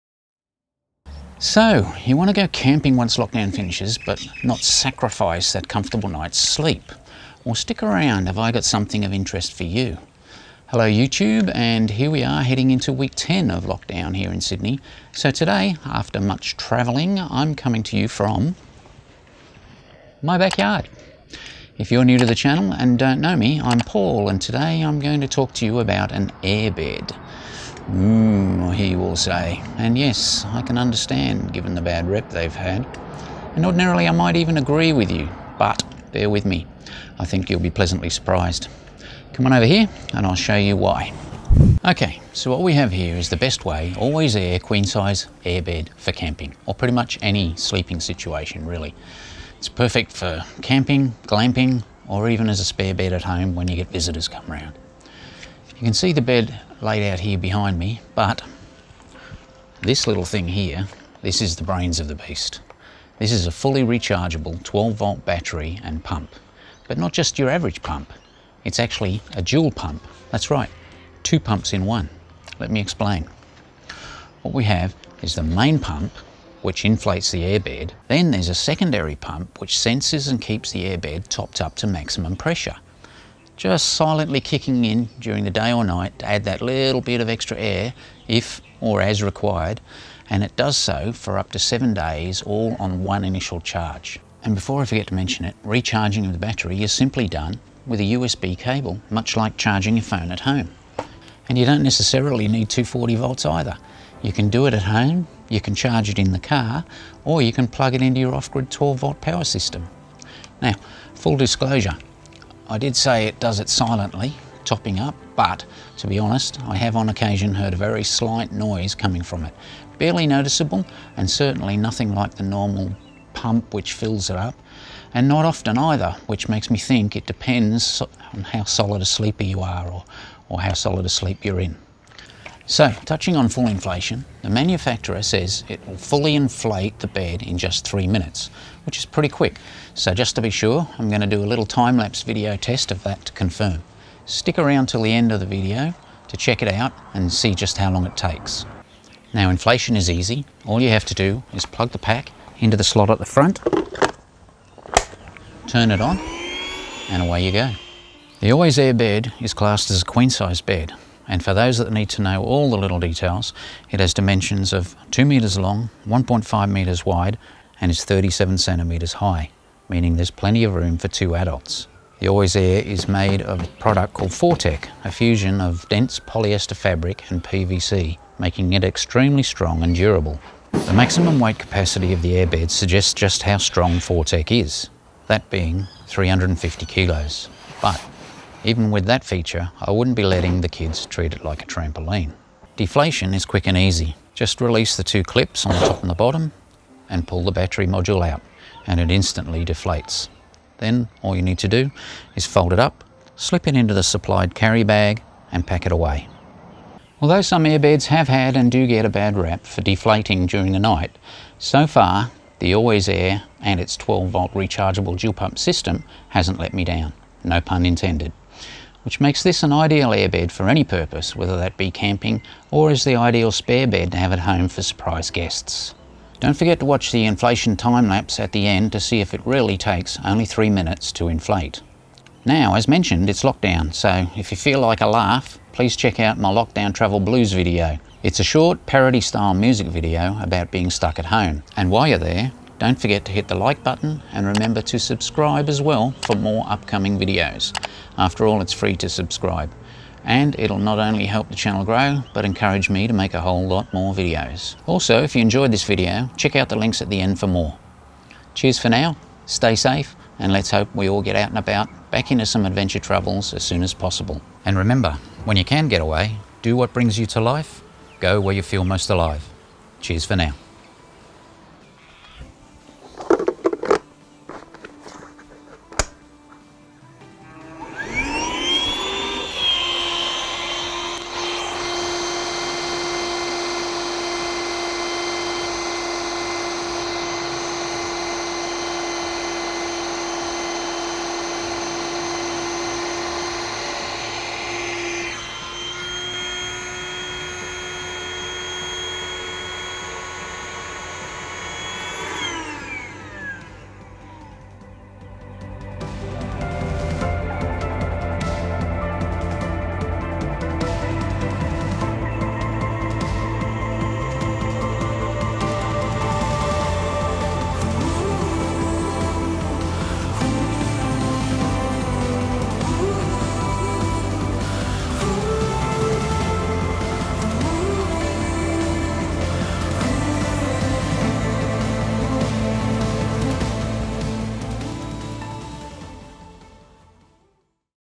Bestway AlwayzAire 12 Volt Rechargeable Dual Pump Airbed for Camping
The awesome Bestway Alwayzaire 12 Volt Rechargeable Dual Pump Airbed will let you have the best nights sleep when camping. It's also great for those last minute visitors that need somewhere to stay. The rechargeable dual pump system will inflate and top up (yes, top up) the airbed for 7 days all on just one charge.